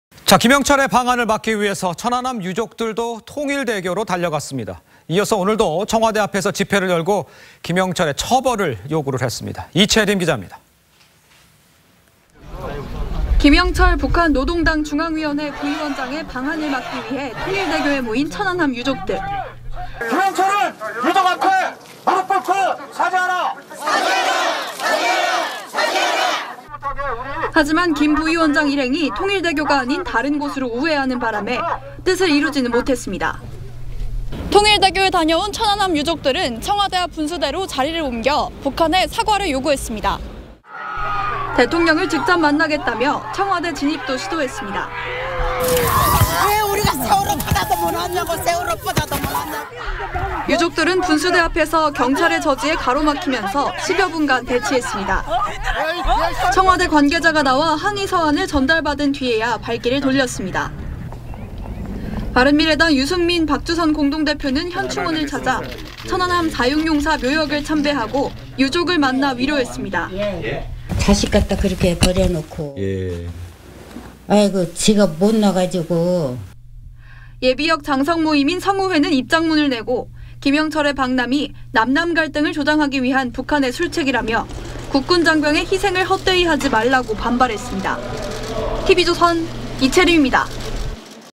"김영철 유족에 사과하라"…천안함 유족 청와대 항의 시위
김영철의 방한을 막기 위해 천안함 유족들도 통일대교로 달려갔습니다. 이어 오늘도 청와대 앞에서 집회를 열고 김영철의 처벌을 요구했습니다.